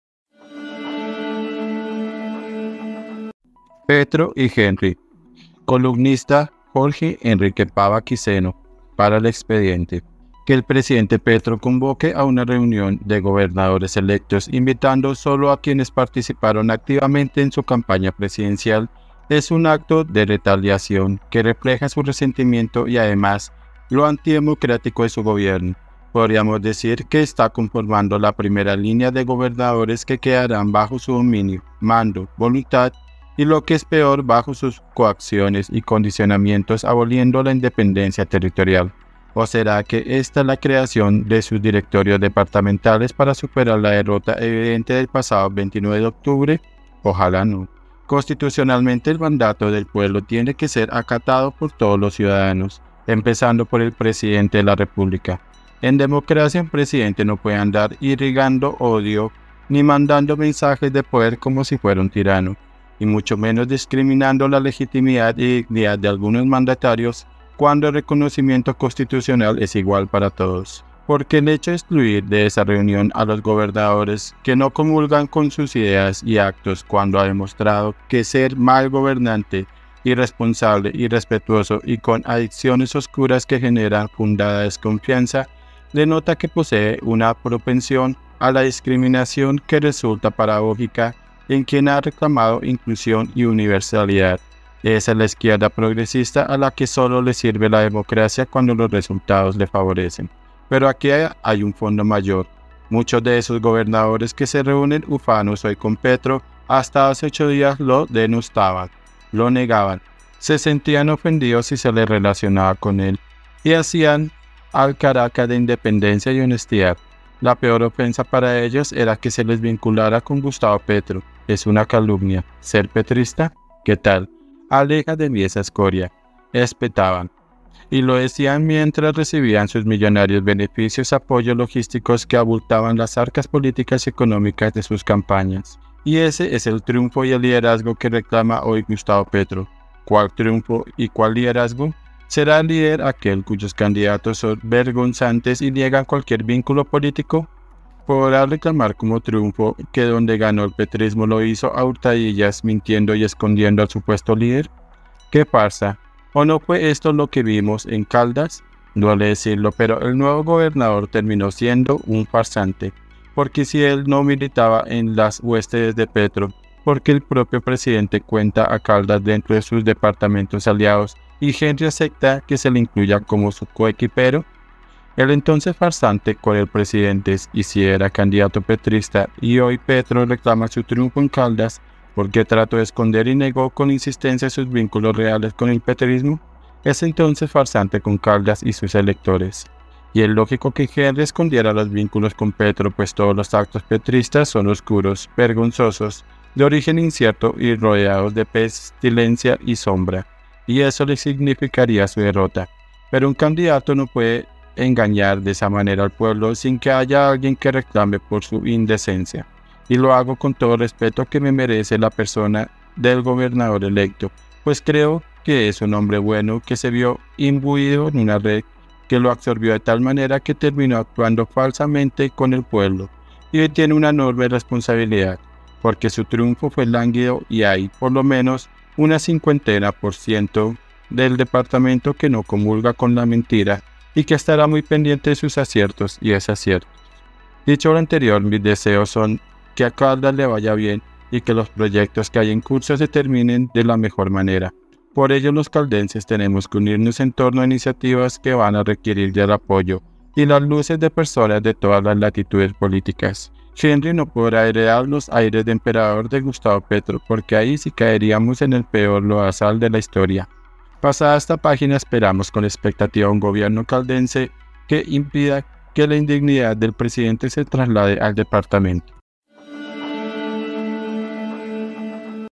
PETRO Y HENRY ⛔ Por Jorge Enrique Pava Quiceno
Que el presidente Petro convoque a una reunión de gobernadores electos, invitando solo a quienes participaron activamente en su campaña presidencial, es un acto de retaliación que refleja su resentimiento y, además, lo antidemocrático de su gobierno.
columna JORGE ENRIQUE PAVA QUICENO